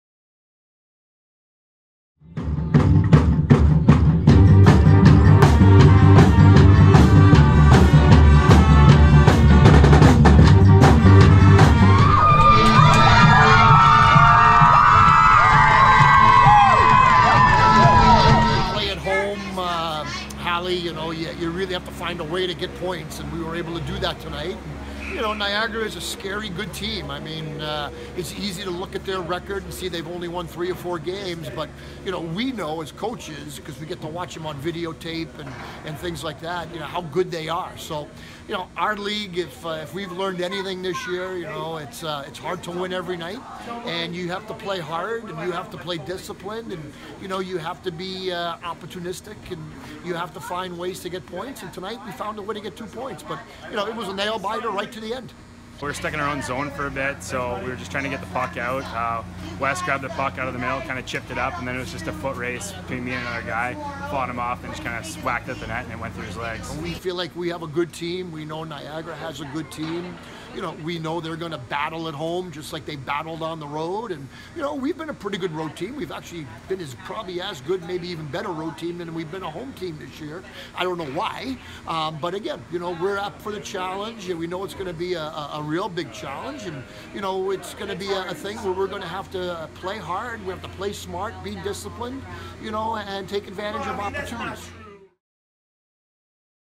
Lakers Hold off Niagara to Win 3-2
The Mercyhurst men's hockey team defeated Niagara 3-2 on Friday January 27. Goals from Zach Todd, Wes Baker and Stephen Hrehoriak. Interviews with Head Coach Rick Gotkin and Stephen Hrehoriak.